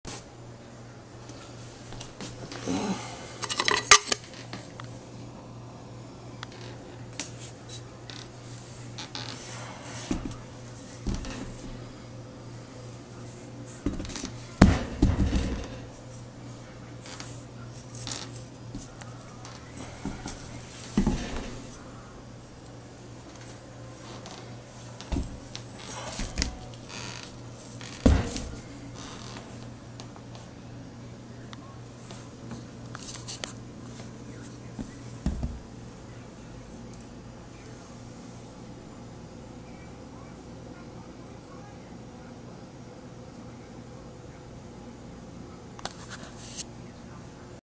Garrett Turbo... maybe M43
What turbo do you think this is, and is it worth the time putting on my 5.9 cummins?